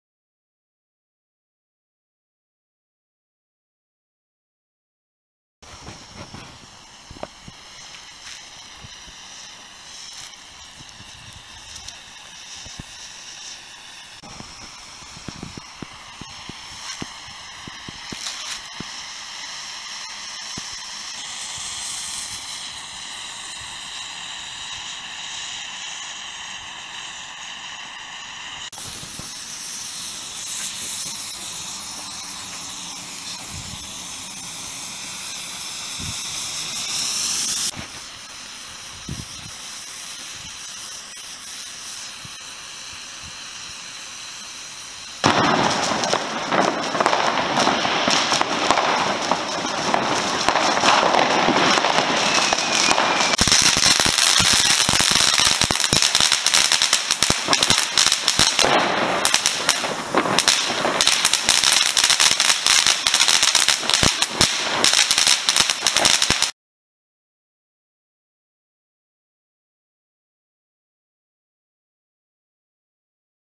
Bengaalse vuurwerkshow
Peter van der Sluijs maakte deze video van zijn Bengaalse vuurwerk hij ontwerpt zelf zijn vuurwerkrekken en standaards